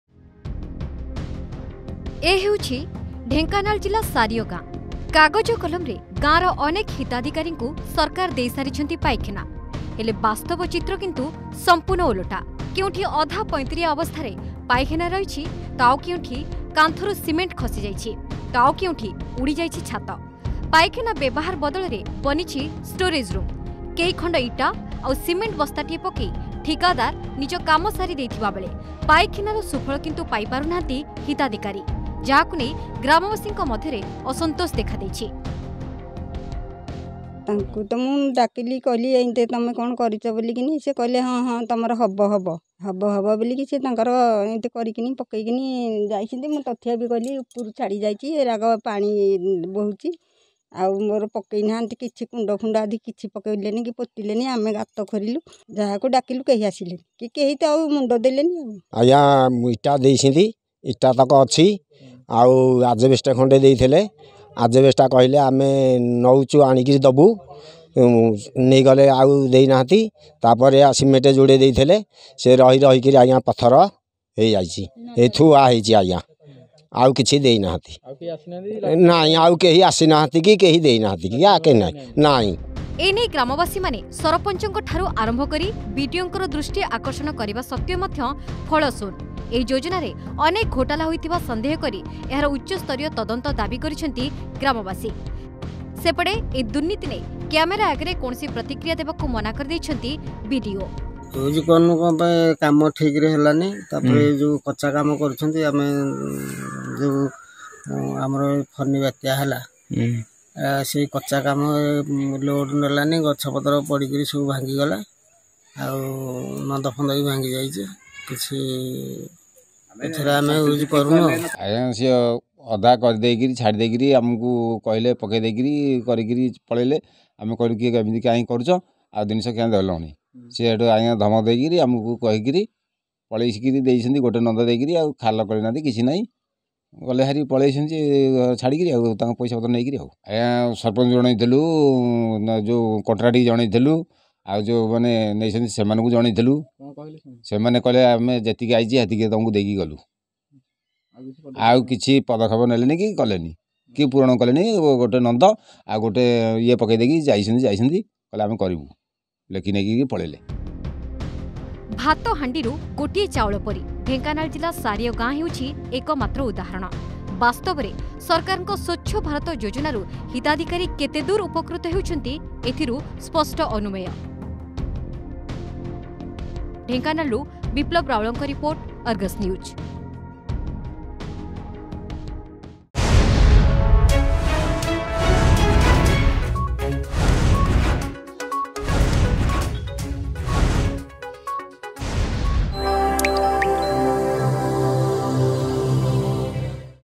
ଢେଙ୍କାନାଳରେ ଫେଲ ମାରିଲା ସରକାରୀ ଯୋଜନା .....
ବାହାରେ ମଳମୂତ୍ର ତ୍ୟାଗ ଦ୍ବାରା ପରିବେଷ ଯେଭଳି ଦୂଷିତ ନ ହେବ ସେ ନେଇ ସରକାର ଆରମ୍ଭ କଲେ ସ୍ବଚ୍ଛ ଭାରତ ଯୋଜନା । ଯୋଜନା ମାଧ୍ୟମରେ ହିତାଧିକାରୀଙ୍କୁ ପାଇଖାନା ତ ମିଳିଲା କିନ୍ତୁ ତାହା କେତେଦୂର ବ୍ୟବହାର ଉପଯୋଗୀ ଢେଙ୍କାନାଳ ଜିଲ୍ଲା ସାରିଅ ଗାଁ କୁ ଦେଖିଲେ ସ୍ପଷ୍ଟ ବାରି ହୋଇପଡୁଛି .